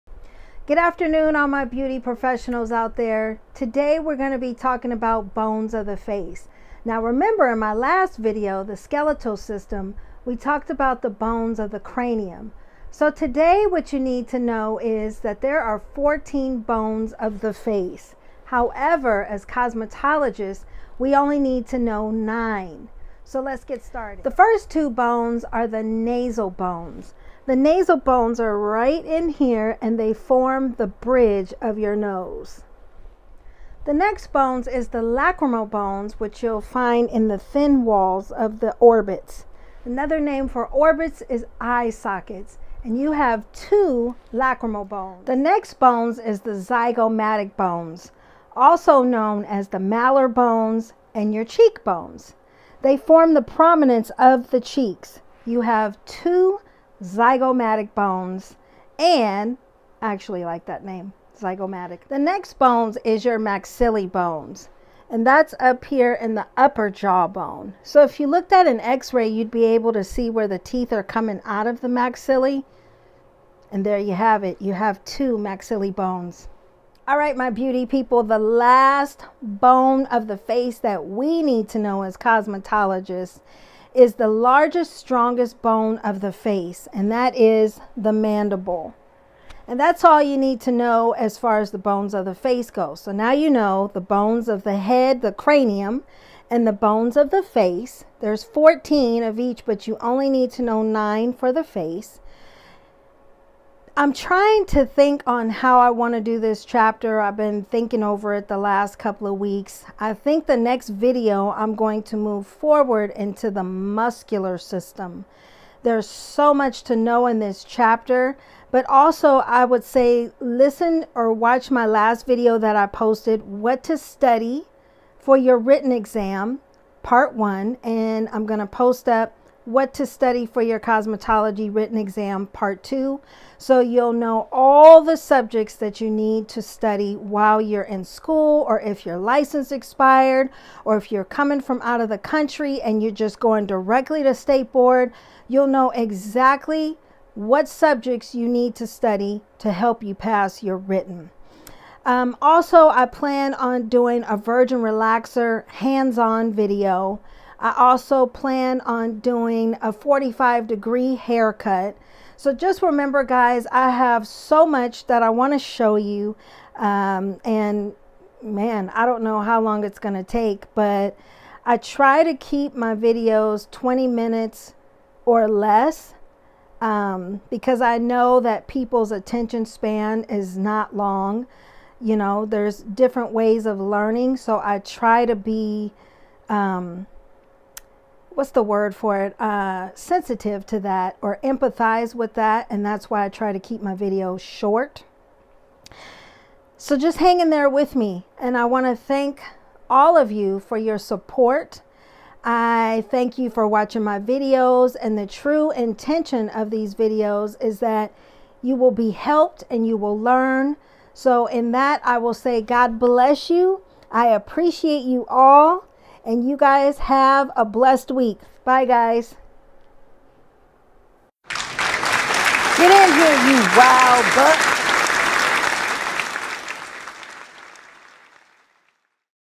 Anatomy and Physiology (Bones of the face)
Continuance from the bones of the head. Chapter 6 in the Milady Textbook. As cosmetologists we only need to know 9 of the 14 bones of the face. Enjoy! Be sure to watch my last video What to study for your cosmetology written exam pt.1 which is from my podcast. Also watch the last video on the skeletal system. God bless. 🙏🏽❤️